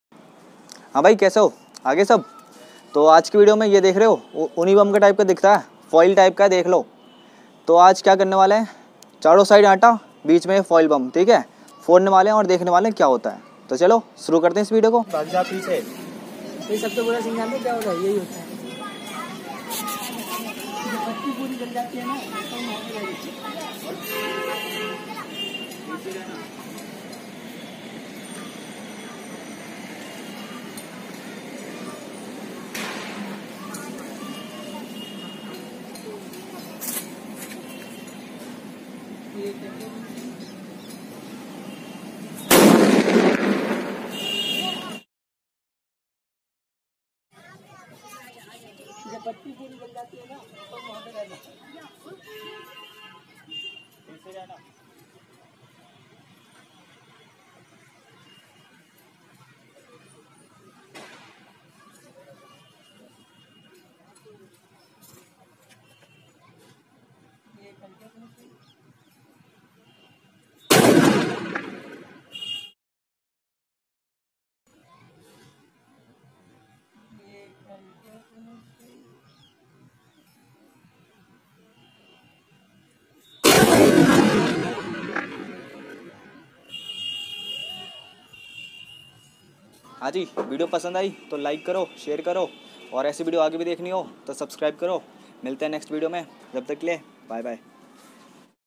Hydra Foiled Bomb and Atta (Wheat flour) | Diwali Experiment | By Experiment Adda
In this video we are going to burst foiled Hydra bomb which is also known as sutli bomb, green bomb and Nazi bomb with wheat flour aka Atta. It will produce huge lightning with great sound.
Team Experiment Adda

Team Experiment Adda